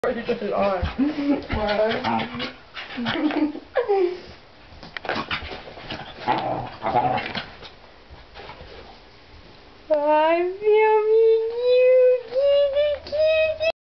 My Scottish terrier Holly :)
My dog goes crazy after a bath.